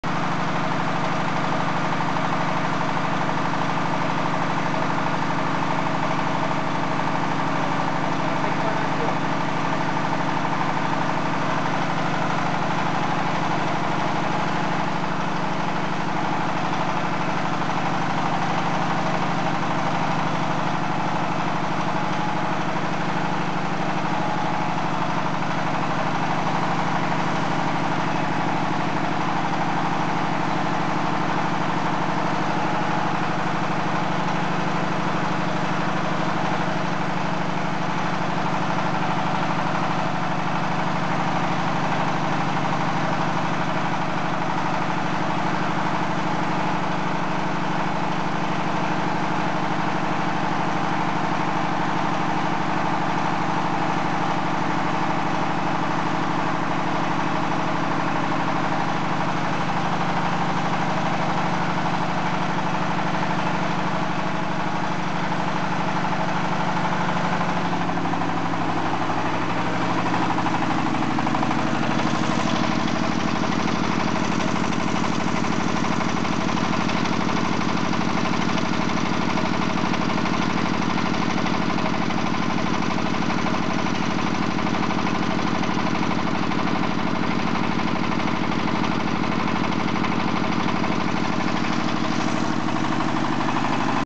XY 1A Crawler Type Drilling Rig
Core Drilling Rig (Engineering Drilling Rig)
XY-1A Crawler Type Drilling Rig can be installed on a crawler and equiped with hydraulic tower and leg.